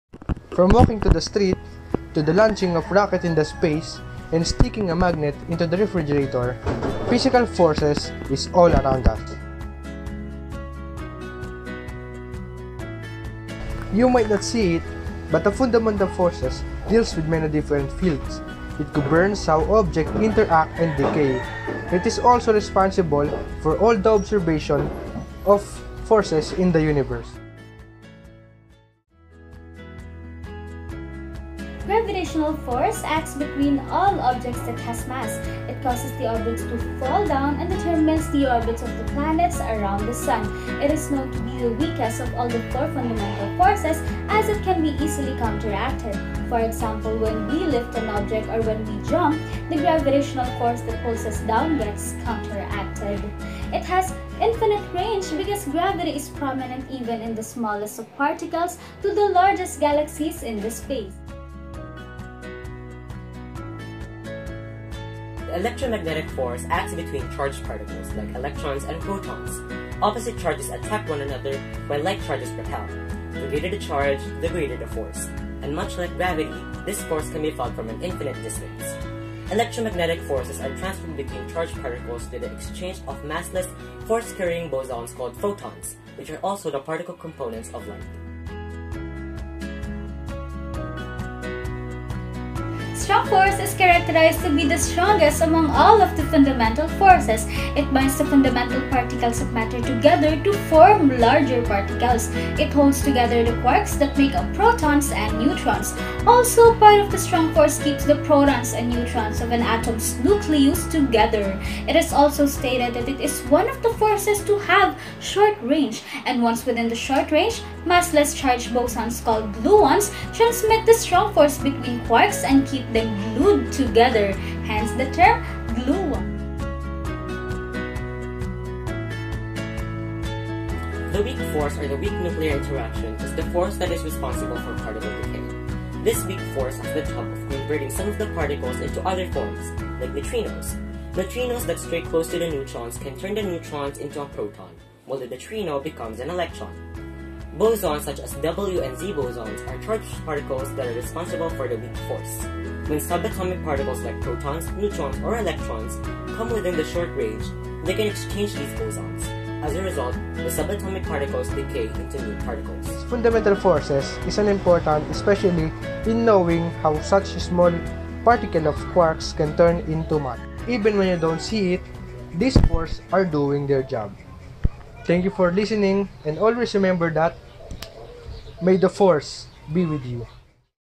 SIYENSIKULA CAYETANO ARELLANO HIGH SCHOOL Videos Four Fundamental Forces
The Video discussed the basic Information about the Four Fundamental Forces of Nature.

Proponents:
Princess Jeza Gail Eliang
John Efraim Junio
Jose Lorenzo Urquico

All Animation from Jose Lorenzo Urquico

Video and Animation Software used: Filmora 9, FlipaClip, CapCut

Credits to Jose Lorenzo Urquico for editing the Thumbnail.